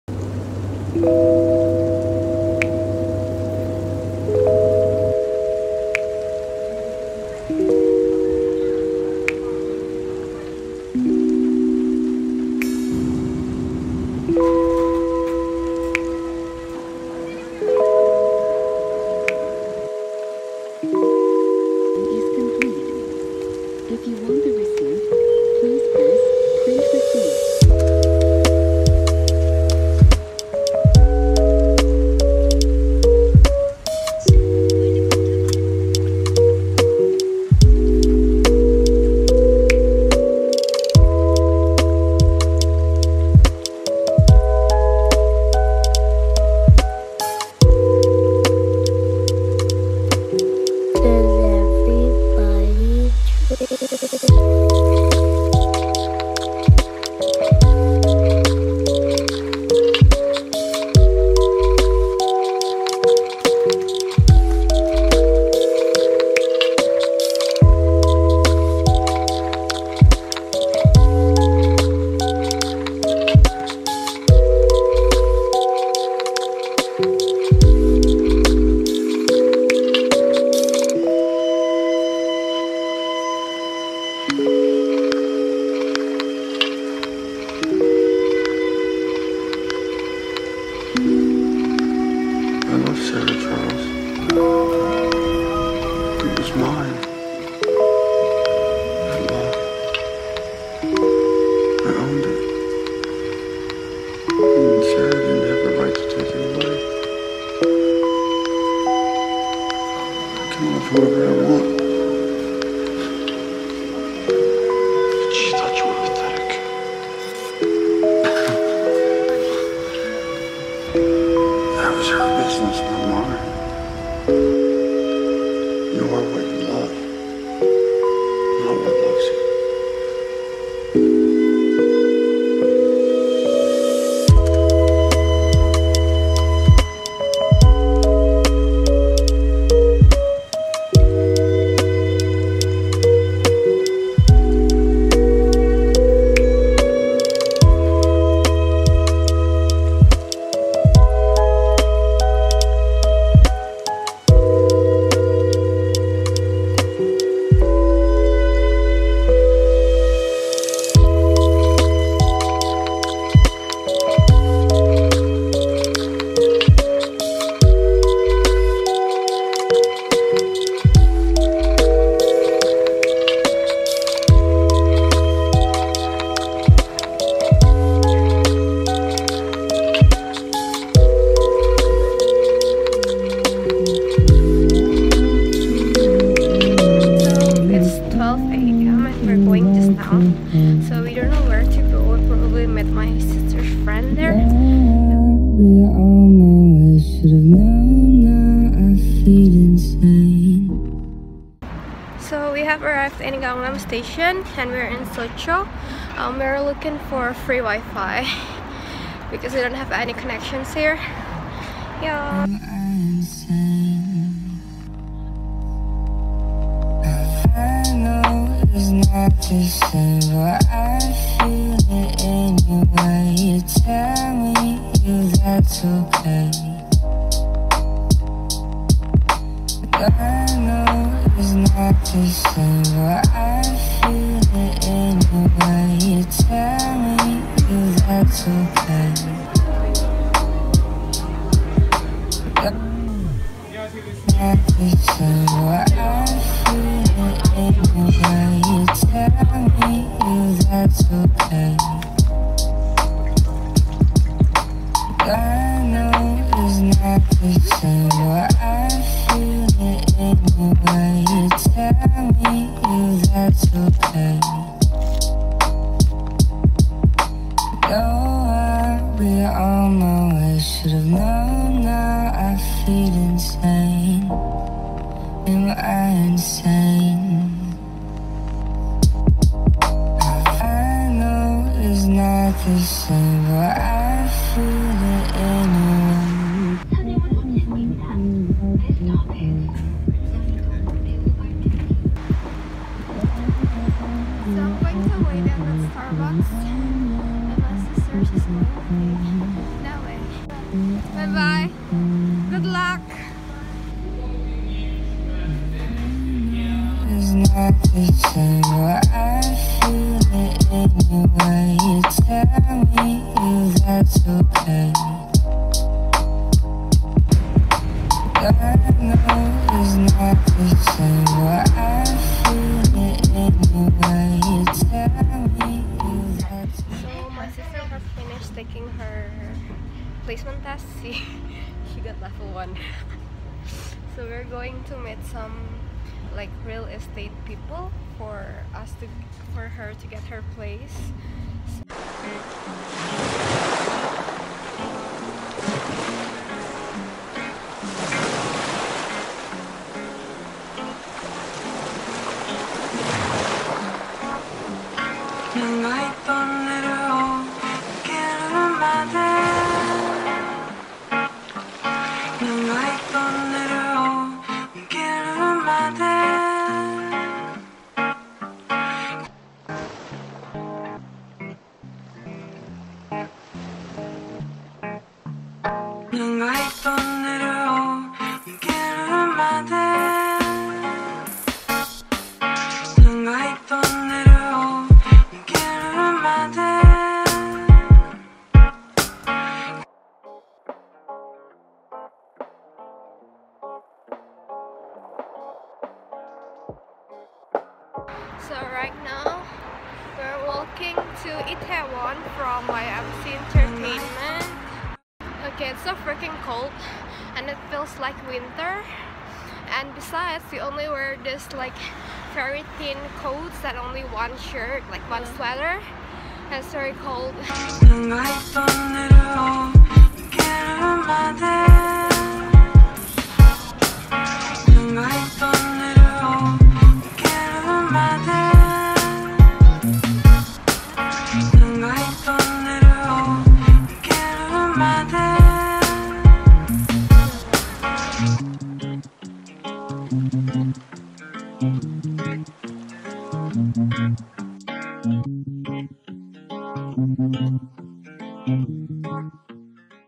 💗 korea vlog (part 1) | korean skincare, wanna one, ymc ent, yonsei | ughbonnie 💗
wow the internet here is so fast. it only takes 2~3 minutes to upload this video WOAHH! im verrrry impressed..

✰ hi everyone~ please enjoy this video!

✰ equipments
﹆ iphone 6s

※ this is not a sponsored video. everything was bought with my own money.